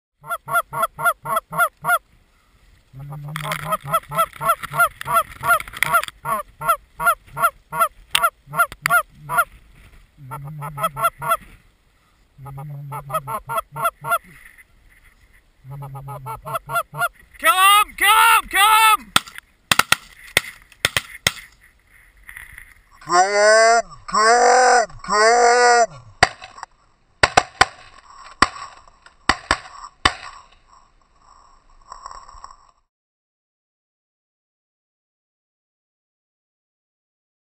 Goose Hunting Lac qui parle with Maxxed Out Guides in Westerm MN
- If these two look like kites coming in they basically were.  30-45 mph NW winds all day on this hunt.